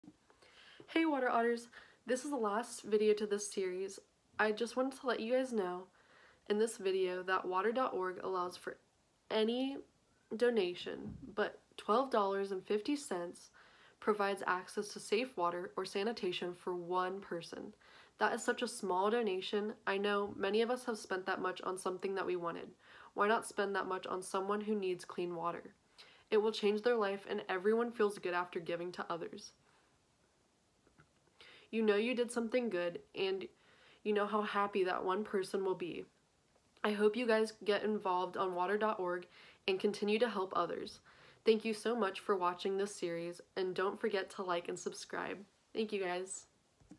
Help people get clean water with $12.50
Talks about donations and how much it helps people live a cleaner and healthier life.